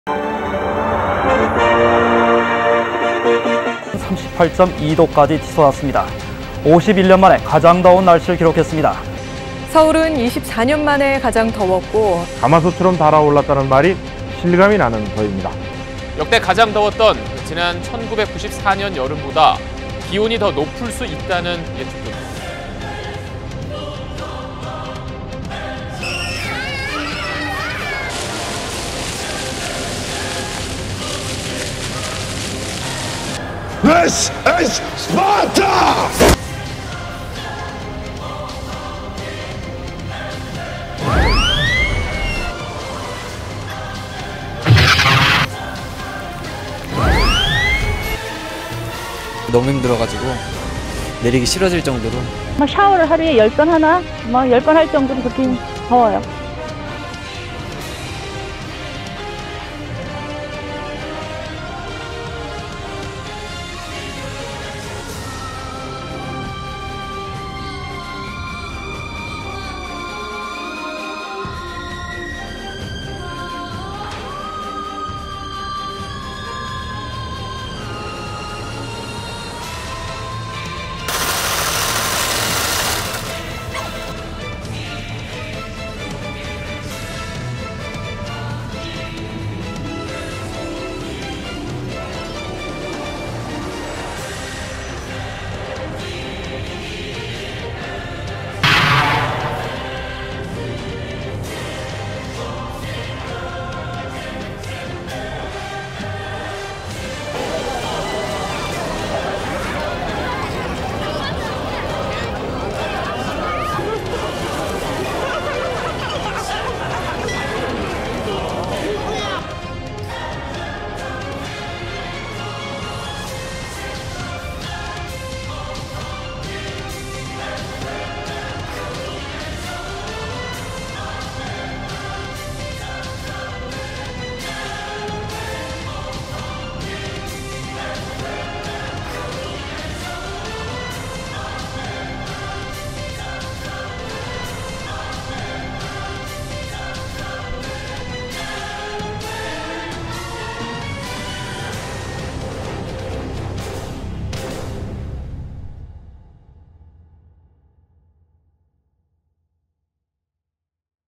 손풍기도 없이 레전드 폭염 견딘 1994년 사람들
역대급 폭염이 찾아왔던 1994년 여름.
당시 사람들은 휴대용 선풍기도 없이 무더위를 견뎌야 했습니다.
열대야를 피해 한강 둔치에 나가 잠을 자는 사람들도 있었습니다.
1994년만큼 덥다는 2018년 여름, 여름을 나는 모습은 어떻게 바뀌었을까요?